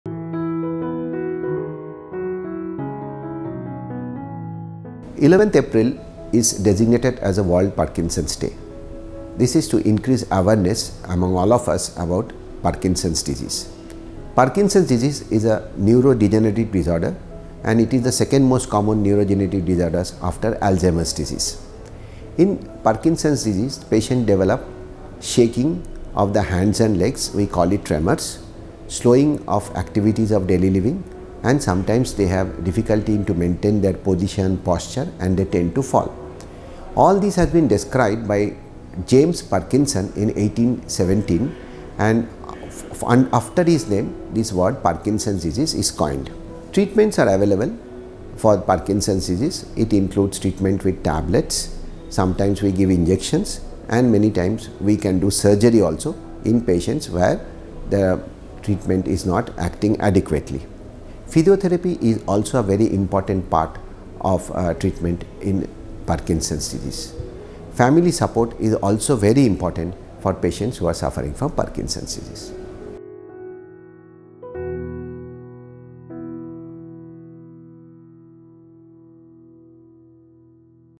What is Parkinson's Disease?
If you have any of these symptoms such as  stiffness of the limbs and trunk, trembling in hands, arms, legs, jaw or head or slowness of movement, you might be developing Parkinson’s disease. Please consult your nearby doctor today!

 On the eve of World Parkinson’s Day, we encourage everyone to create awareness about Parkinson's diseases among the masses. Parkinson is the second most neurodegenerative disorder after Alzheimer.

Dr. Jaydip Ray Chaudhuri, Consultant-Neuro Physician, Yashoda Hospitals discusses about the symptoms and various treatments options of Parkinson's diseases